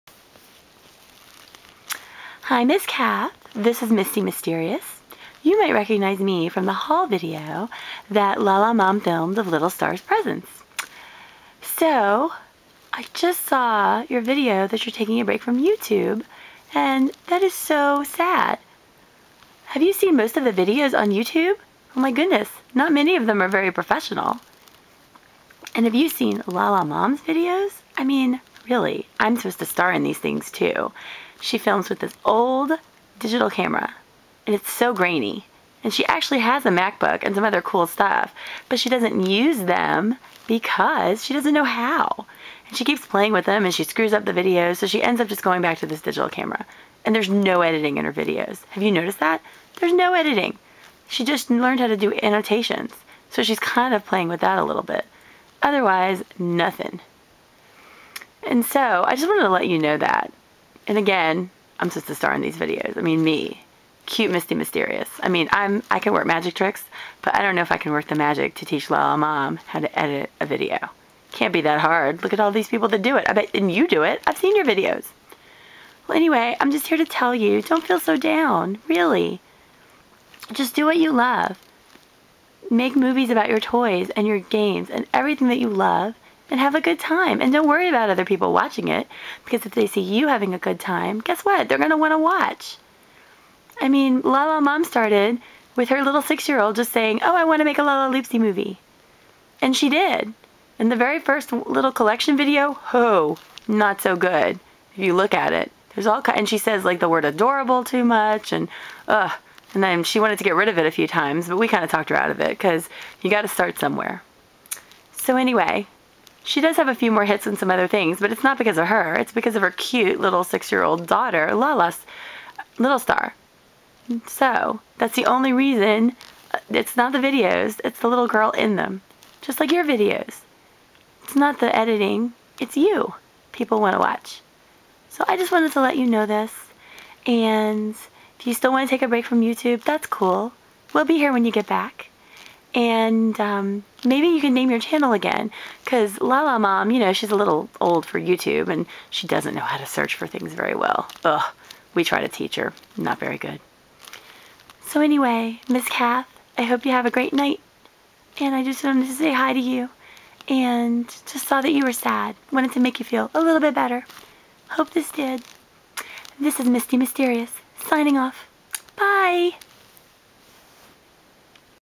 Misty Mysterious' Response to MsKath777
Link to MsKath777's new channel!
Misty Mysterious just had a make a video because she hates to hear anyone sound so sad on youtube. Especially someone with so many good videos! Please cheer up--we can't wait to watch your old and new videos, whatever channel they're on. :-D
xoxoxoxoxo  Lala Mom and Little Star